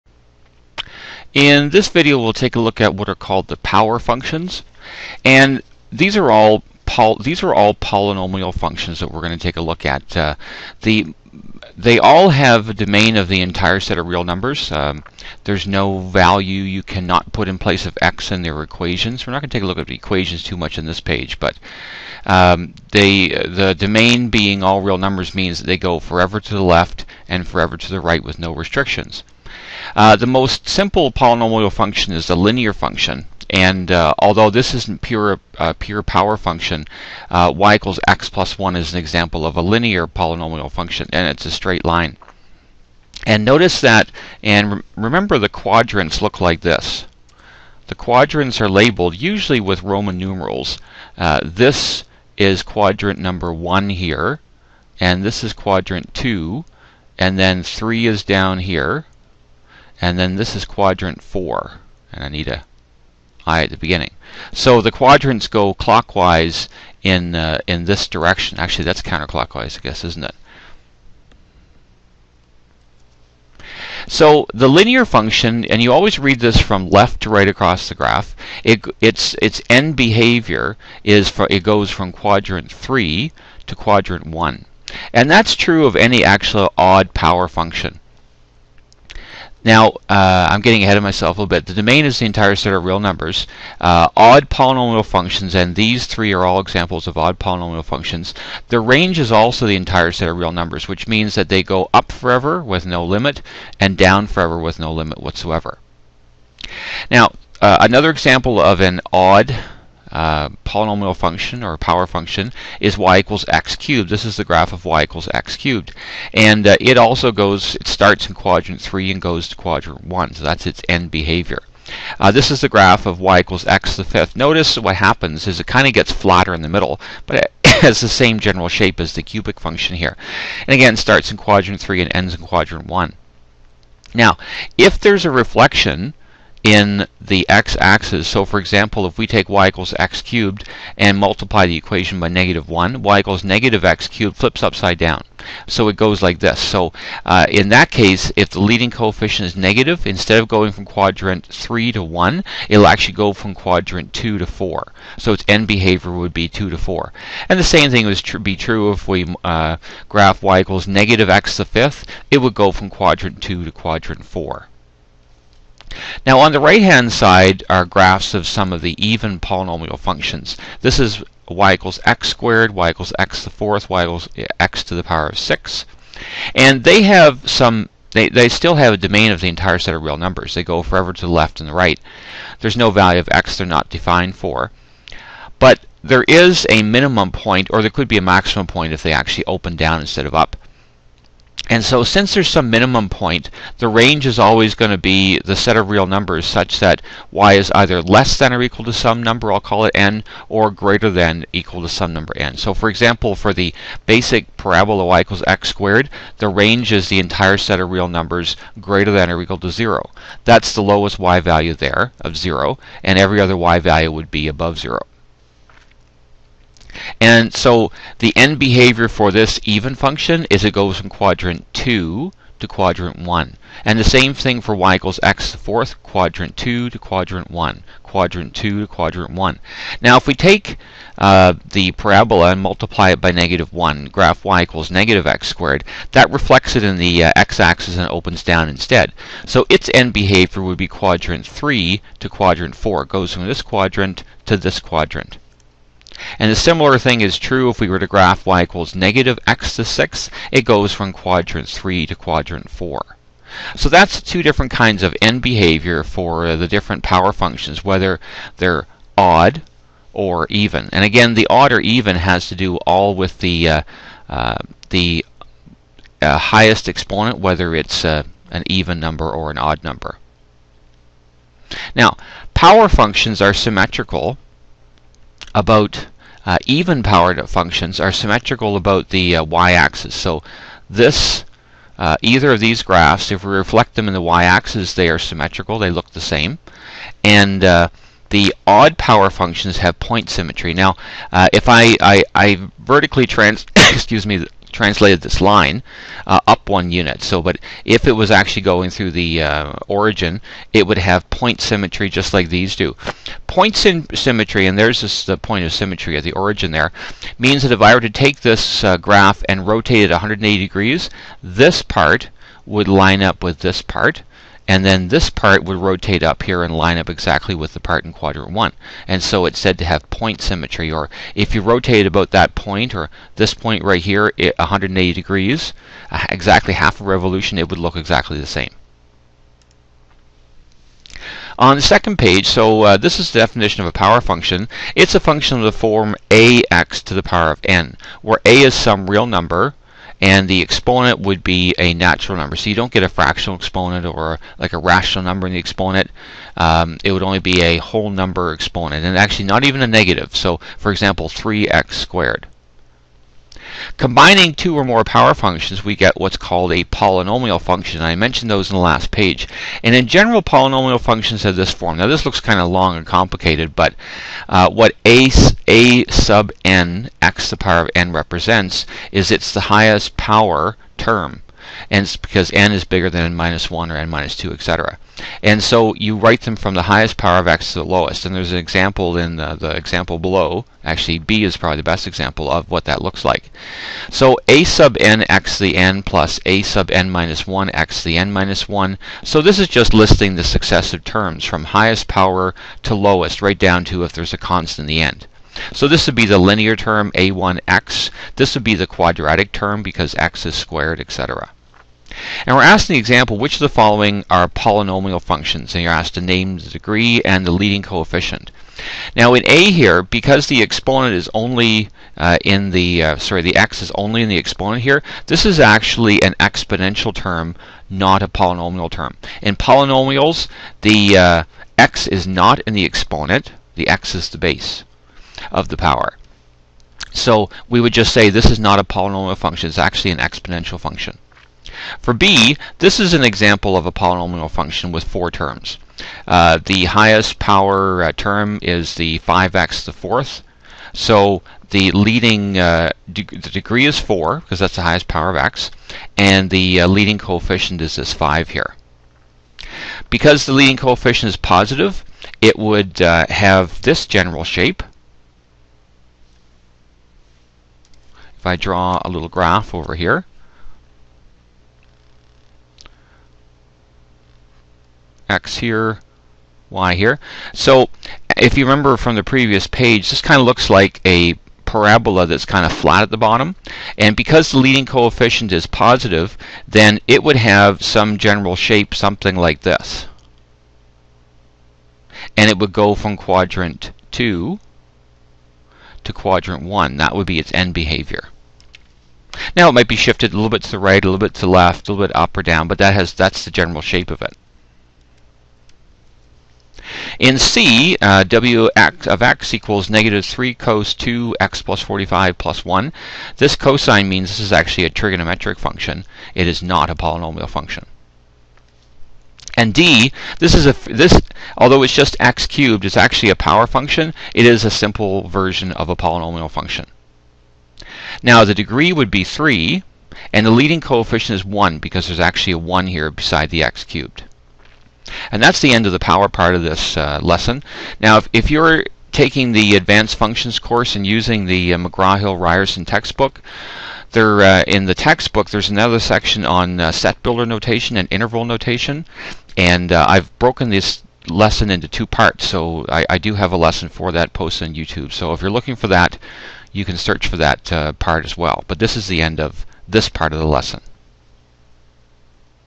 Power Functions
This video shows the basic power functions and what their graphs look like.  General polynomial functions are also discussed and the end behavior of even and odd power or polynomial functions.  This lesson was created for the Advanced Functions (MFH4U) course in the province of Ontario, Canada.